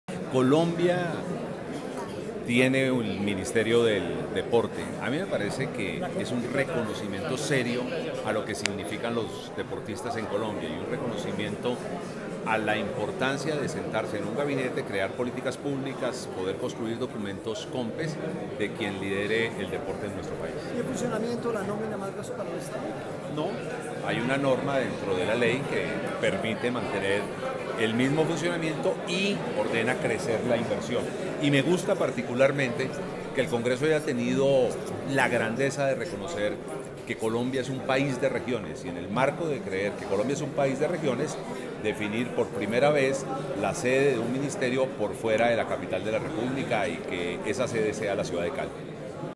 Senador Velasco celebra creación del Ministerio del Deporte que tendría la sede en Cali
Video grabado el día miércoles 21 de noviembre del 2018 en el Salón de apoyo del Senado